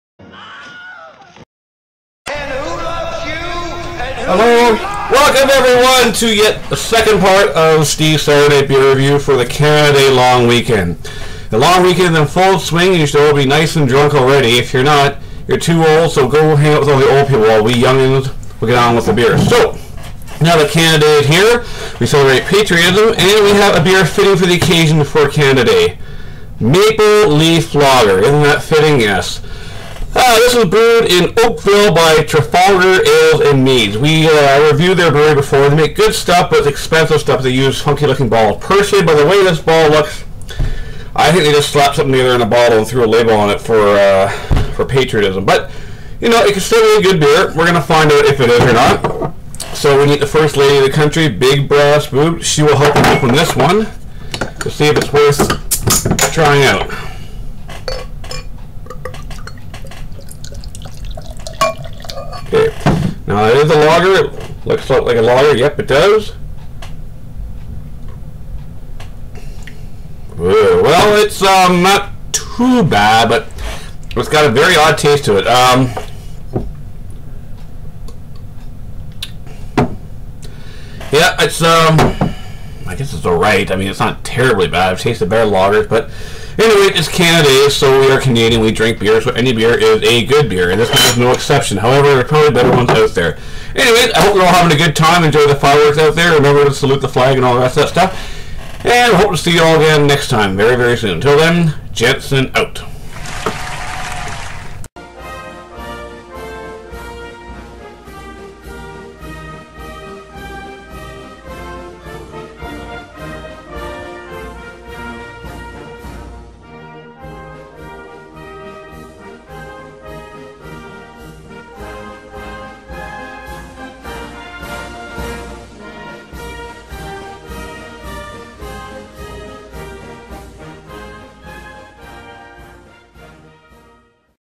Steve's Saturday Beer Review Episode 91
Maple Leaf Lager (Trafalgar Ales & Meads, Oakvill ON Canada)